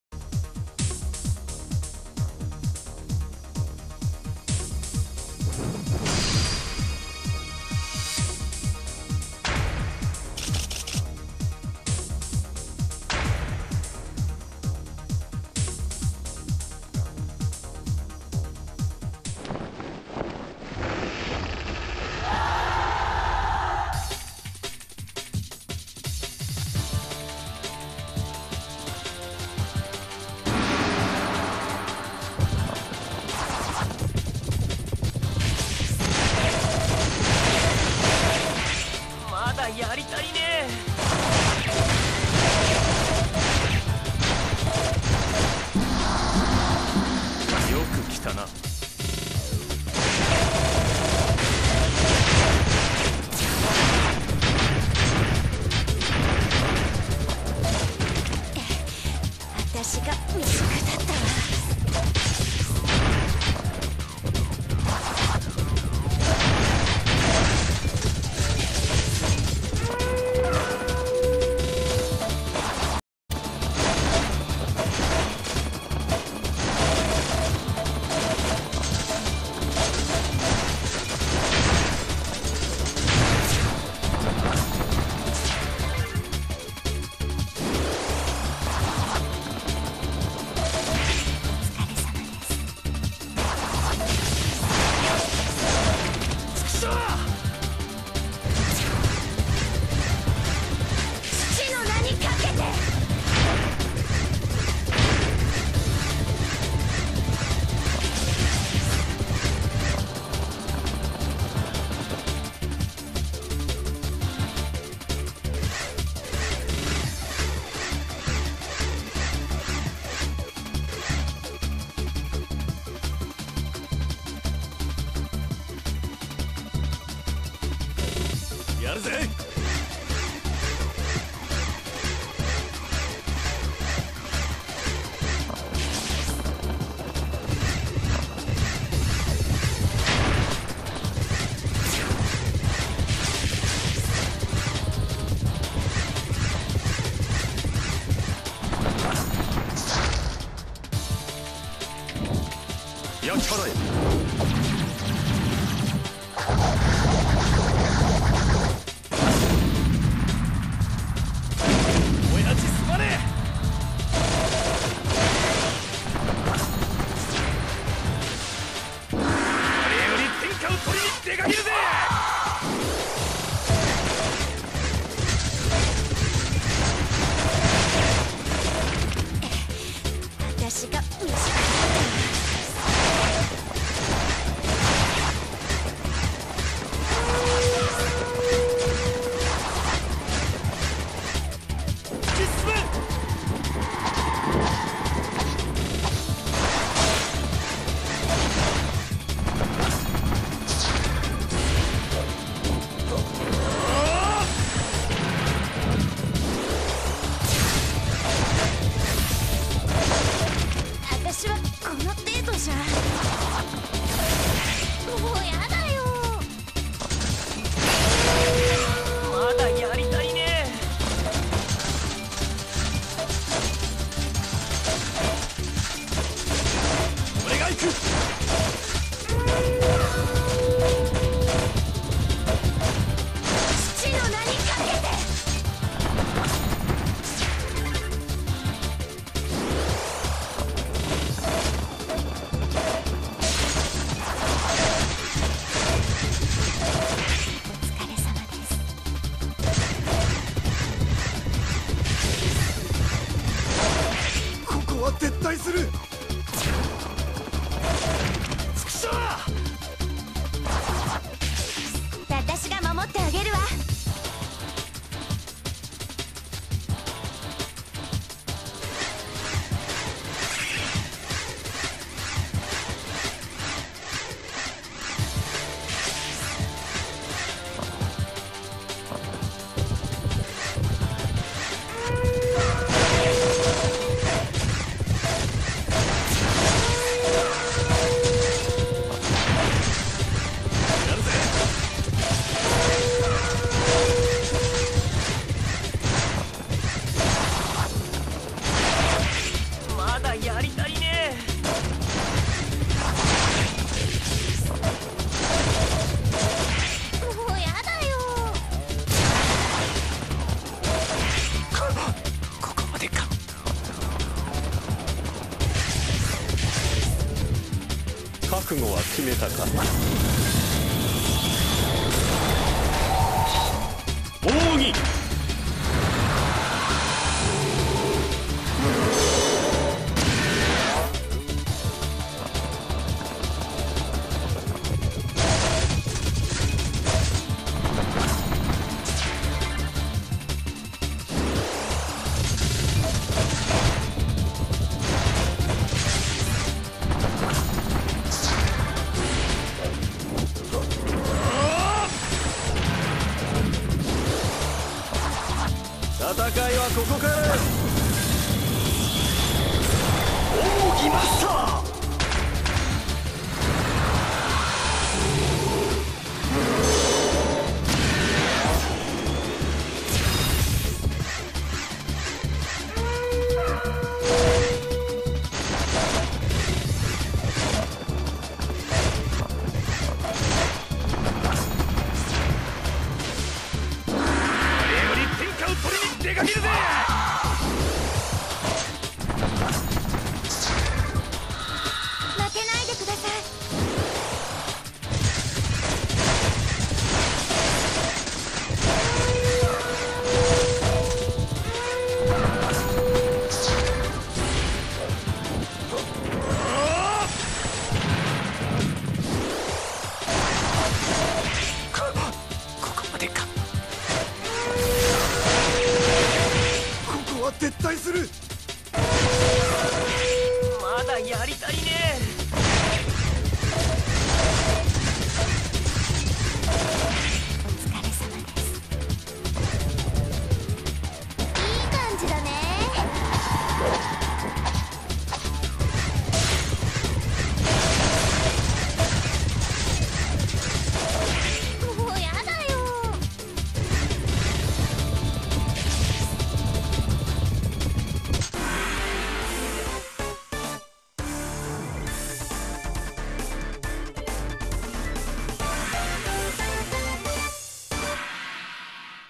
周権ワールド(108)
三連戦(②戦目)
今回はvs赤壁快進撃です。
序盤に孫策を落とせた&柵を壊せたのが良かったと思います。
中盤はﾇﾙﾇﾙとしてましたがなんとか勝てましたｗ
終盤の端攻城はちゃんと劉備が出てれば防げたので勿体無かったです(-_-;)